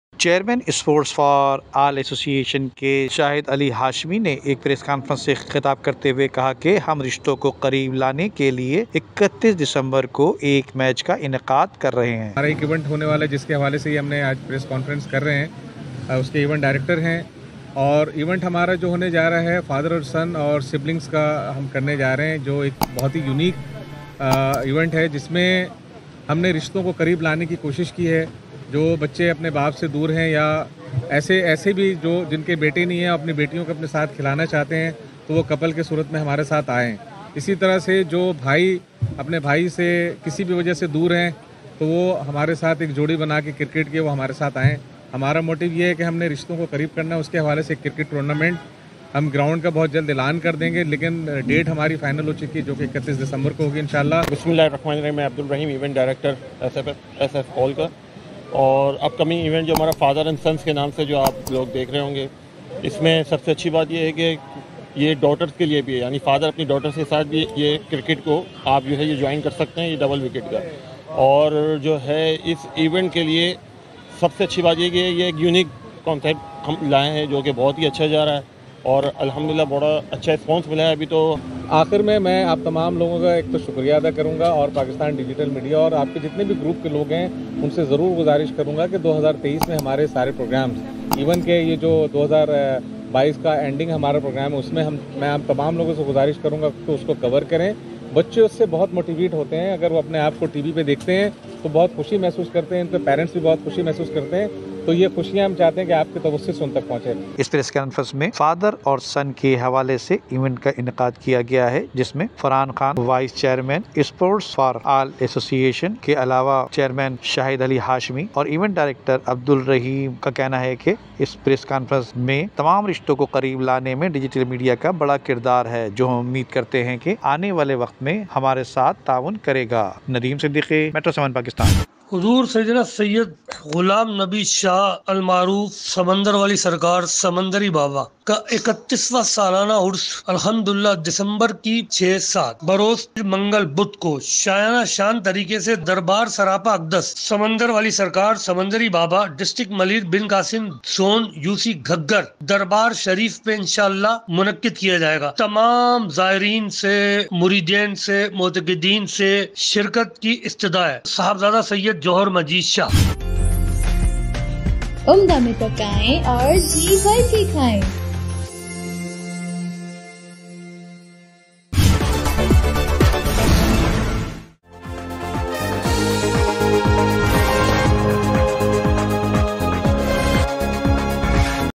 ڈیجٹل میڈیا اسپورٹس ھماری کوریج پر خصوصی توجہ دے چیرمین شاھد ھاشمی
Video from Nadeem